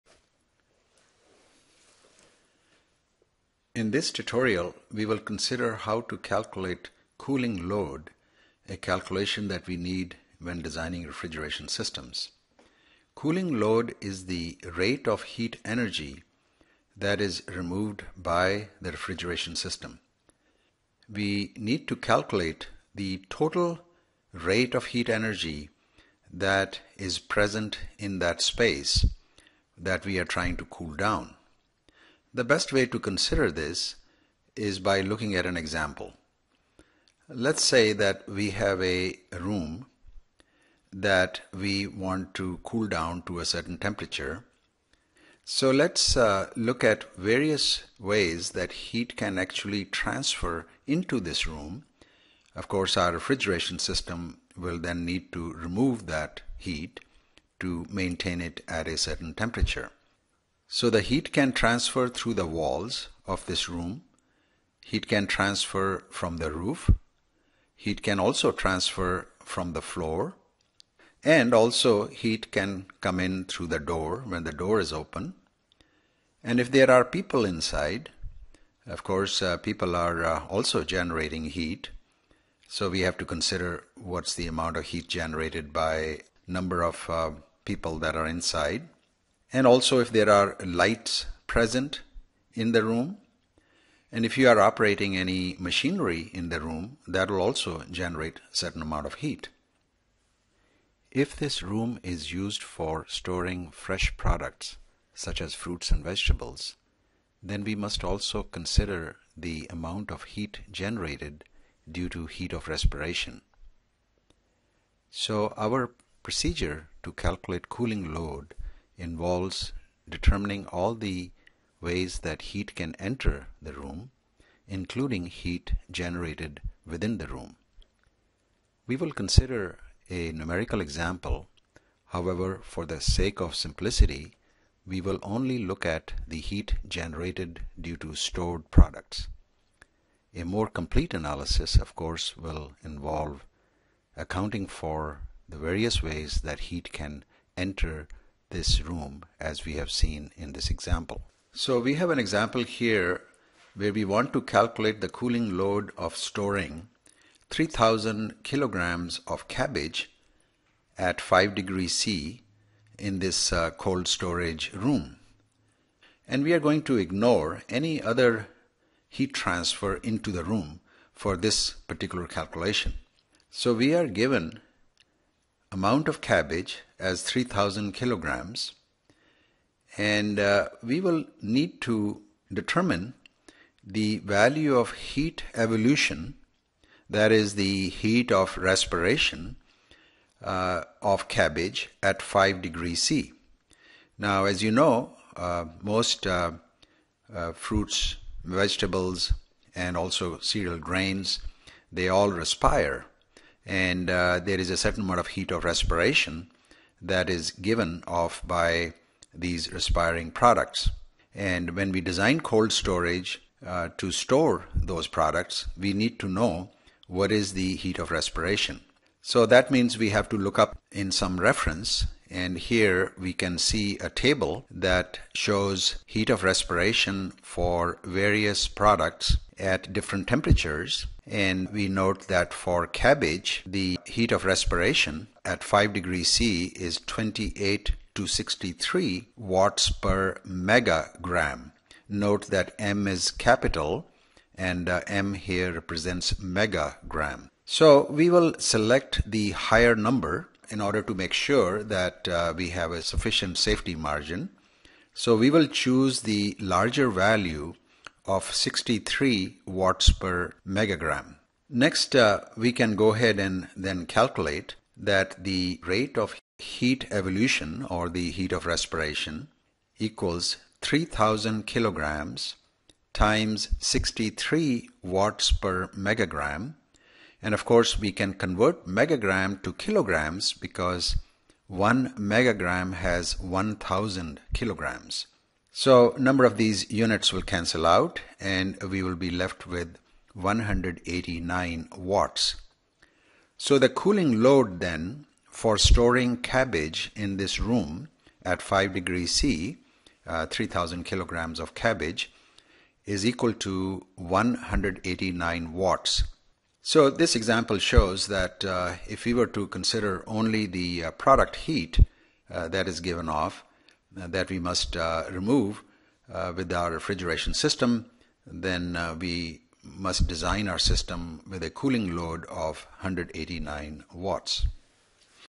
CoolingLoad
Calculation of cooling load used in refrigeration and design of cold storages for foods.